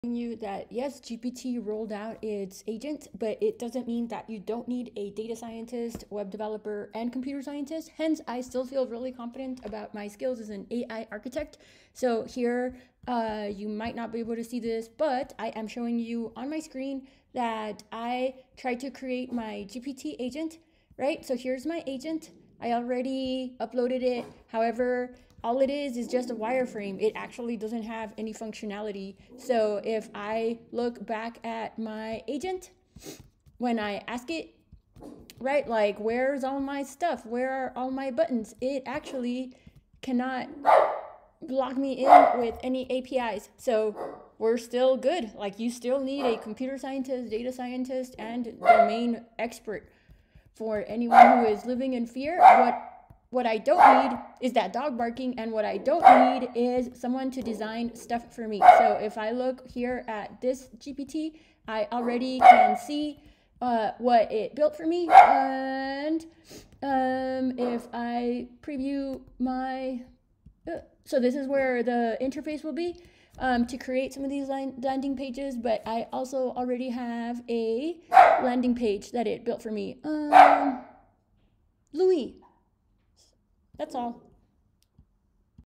still requires
this is how AI is helping thrive today

Showing you that yes I can use the new GPT's announced by openAI, but they are just building wireframes.  So all the superficial digital marketing agencies DID get wiped out, GPT IS able to do that part for us all fairly easily (the thumbnail shows an example), but actually making stuff work still requires someone who is beyond full-stack capable.